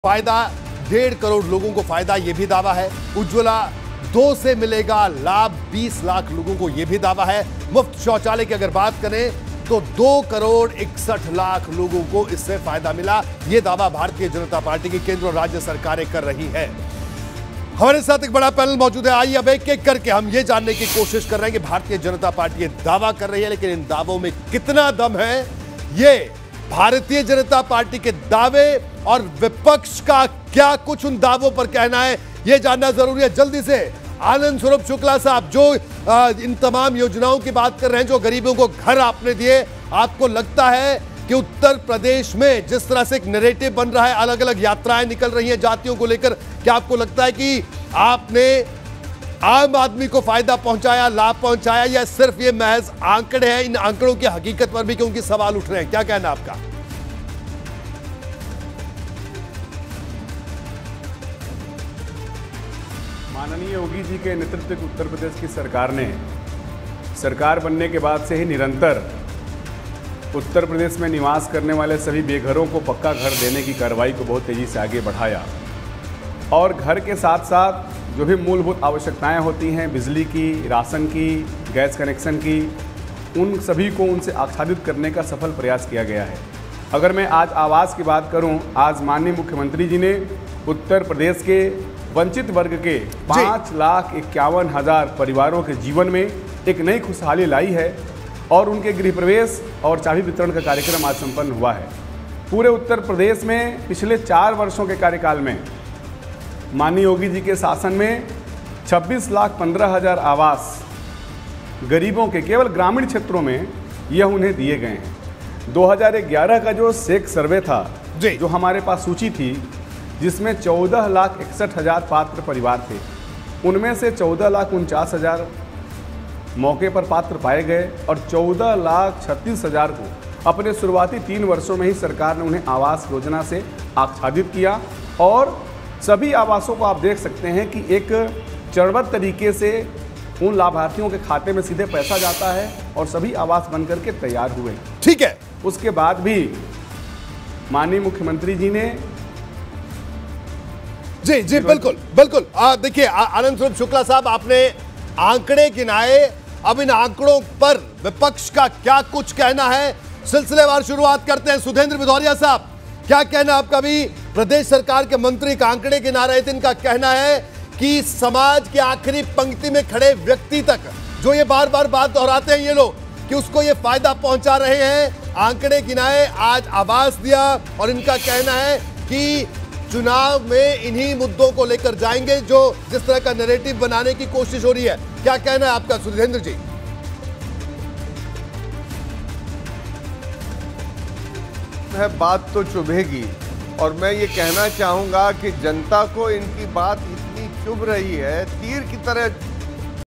LIVE Debate में CM योगी के मंत्री ने किया ग्रामीण आवास पर ये बड़ा दावा | BTC | Hindi News
ग्राम विकास राज्यमंत्री आनंद स्वरूप शुक्ला से जब पूछा गया कि क्या आपने आम आदमी को फायदा पहुंचाया है। इसपर सुनें उन्होंने क्या जवाब दिया।